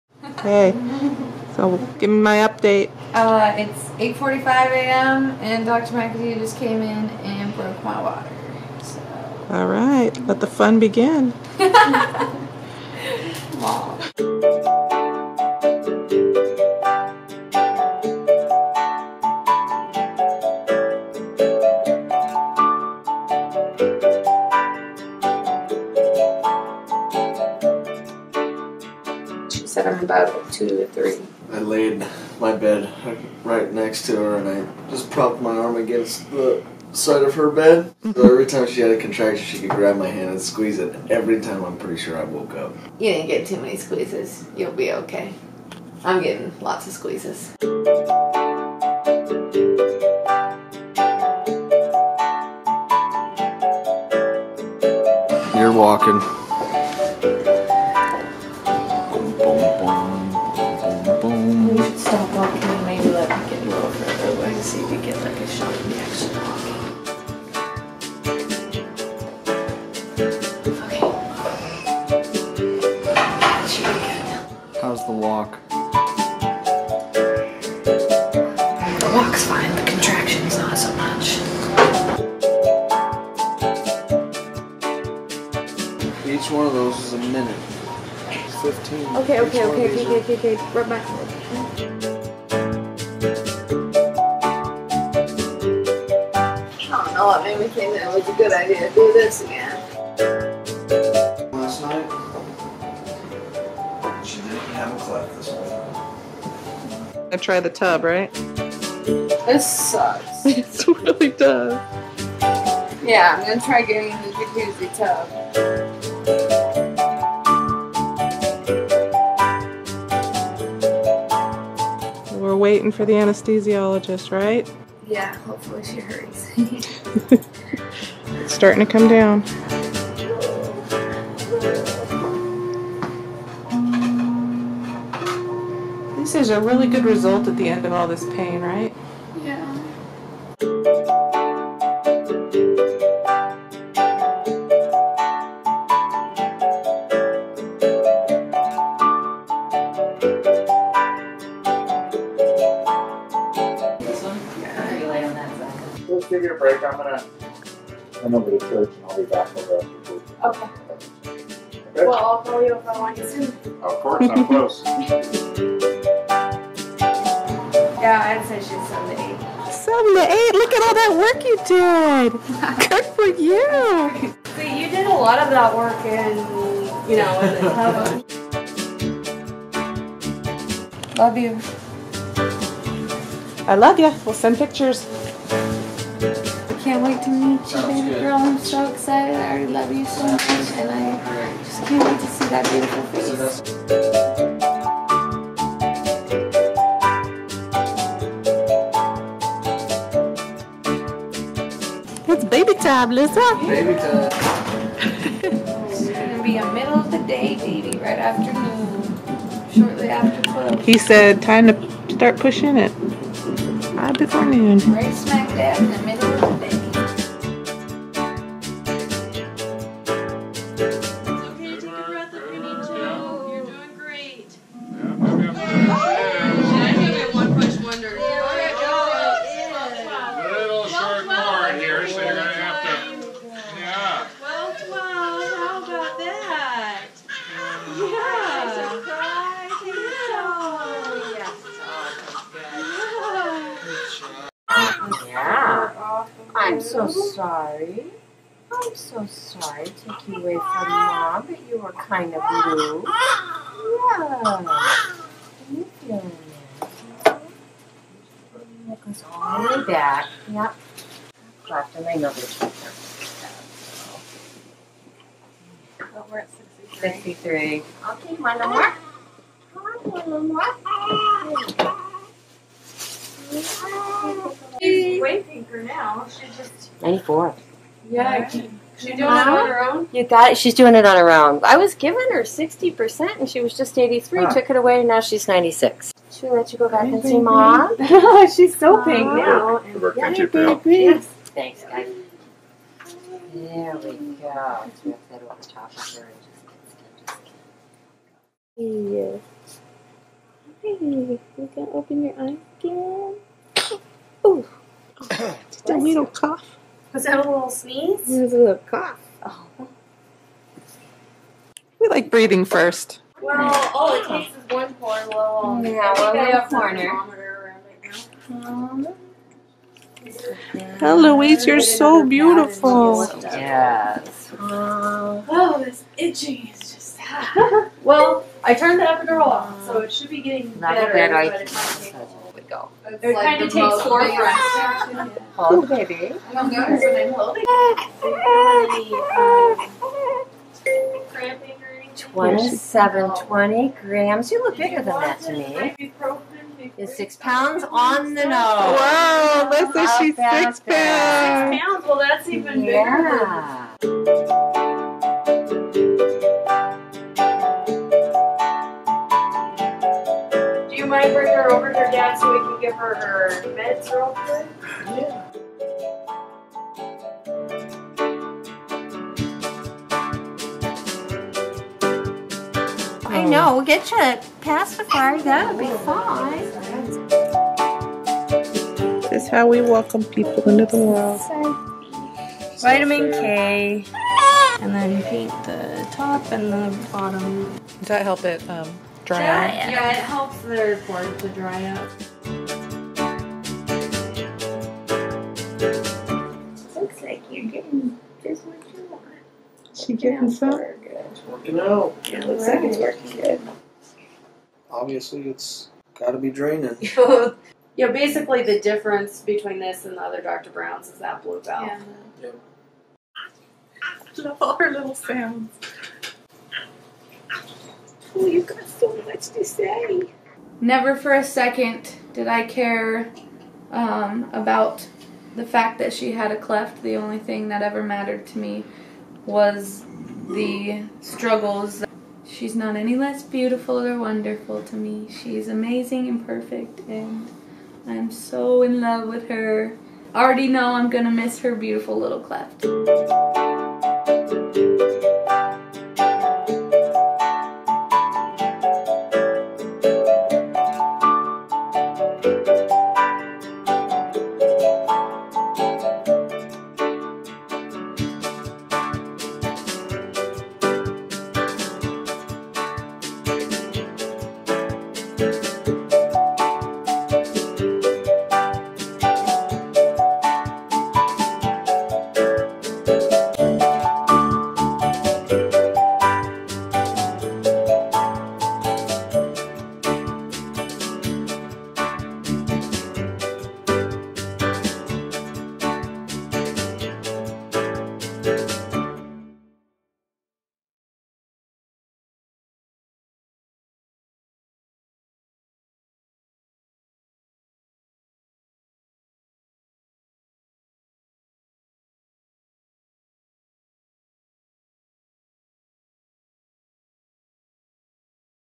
Eloise's Birth
Eloise Nicole Mitchell was born to Joe and Alyssa in April, 2018. She was welcomed by our entire family, and had lots of visitors. Eloise was born with a cleft lip and palate. She has some surgeries ahead, but loving parents to help her through it.